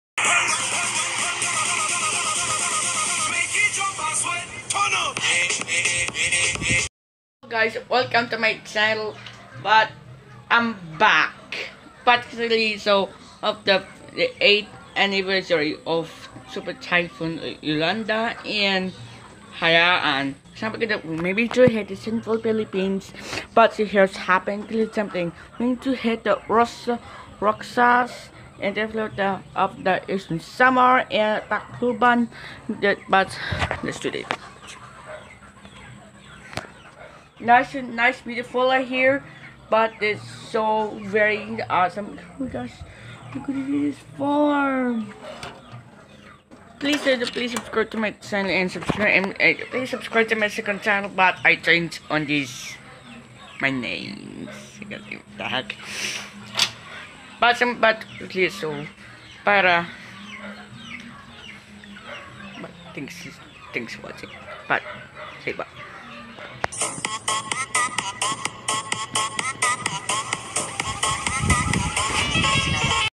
8th Anniversary If Super Typhoon Yolanda And Haiyan (Im Back Guys)
Im Back Guys

8th Anniversary Of Yolanda And Haiyan Hits Central Philippines And Maybe Hit Eastern Samar And Roxas City And Tacloban Yep In November 8 2013 But Here's Happen